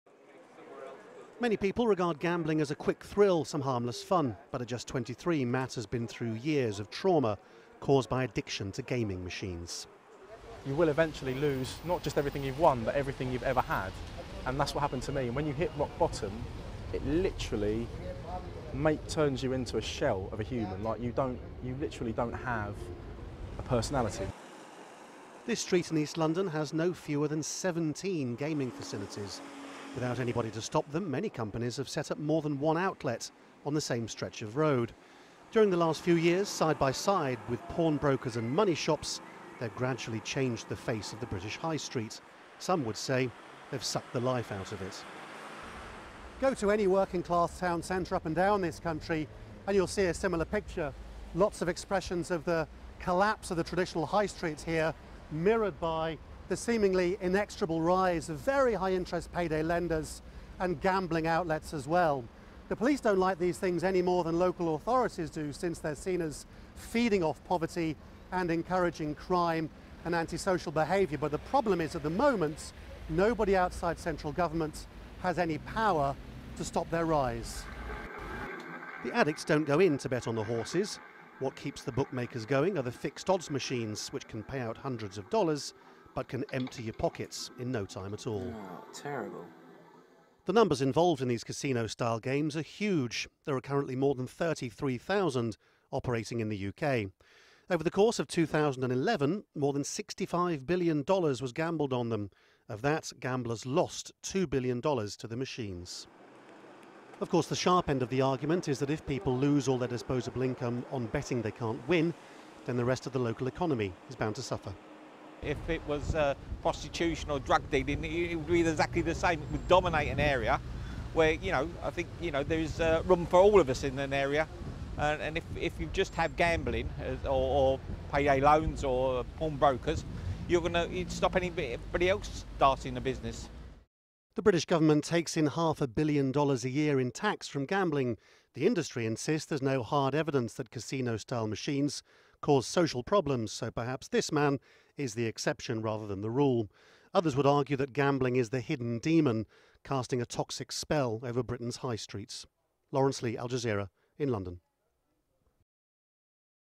Gambling facilities mushroom across London
London is feeling the long term effects of the financial downturn.

Local politicians say the city has up to 7,000 empty shops, costing the economy more than $500m and leading to social problems.

The influence of high street gambling facilities could be making the situation worse.

Al Jazeera's Laurence Lee reports from London.